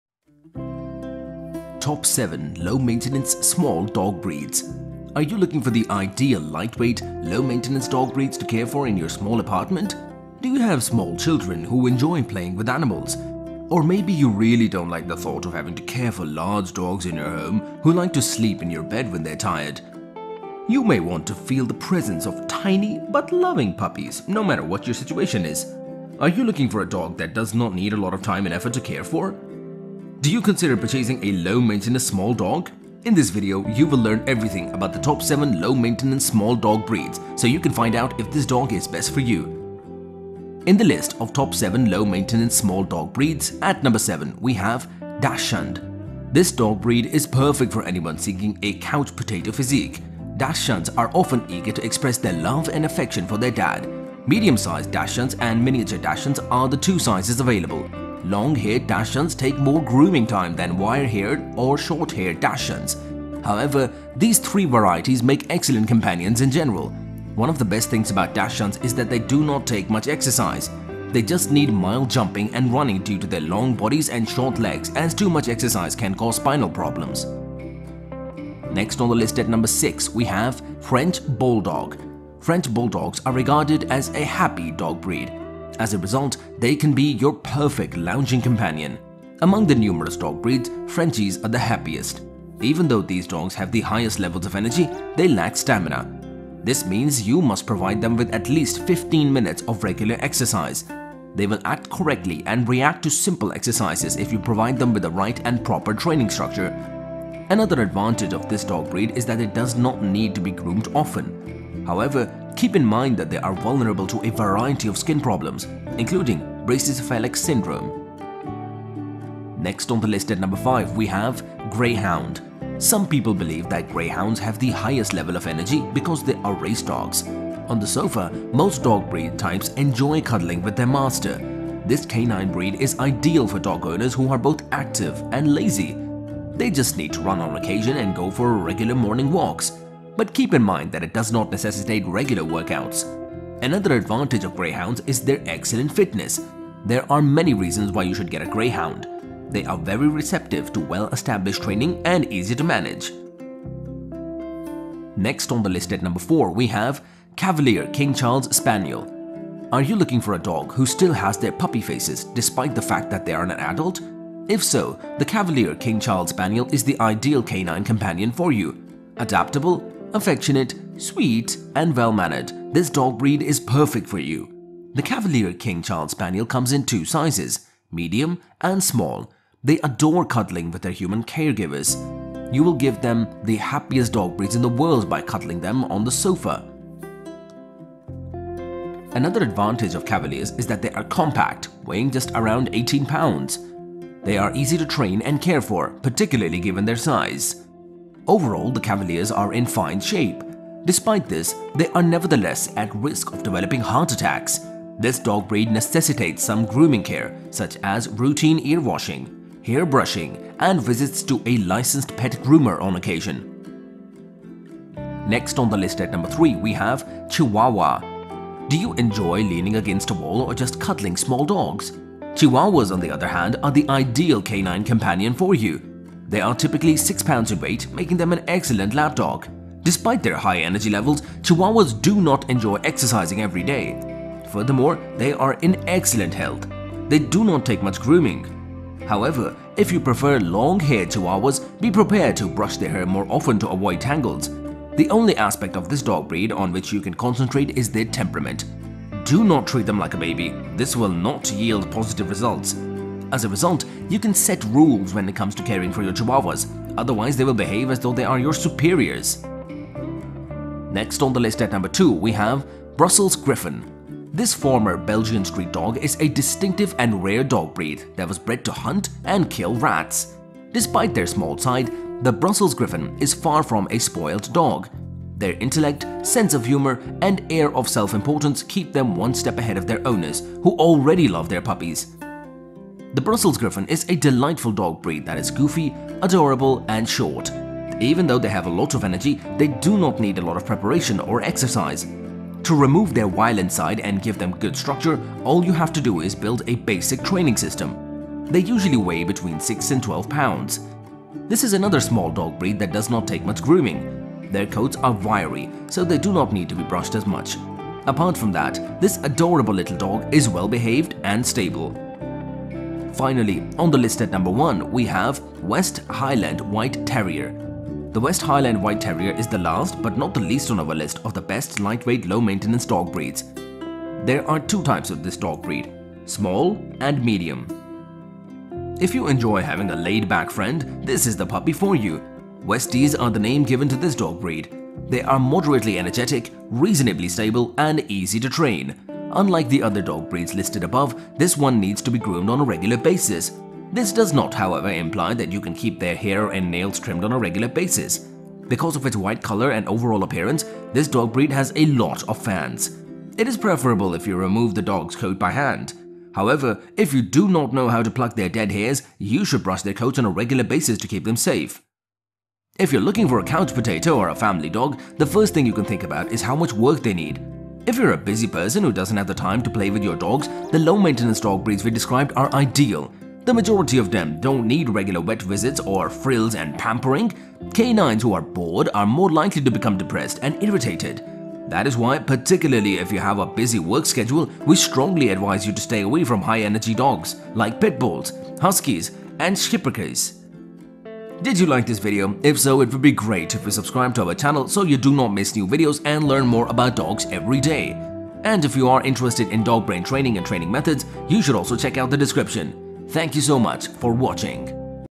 Top 7 Low Maintenance Small Dog Breeds | Dog World
Looking for dog breeds that don't need much maintenance time? Watch this video and learn about the top 10 low maintenance small dog breeds.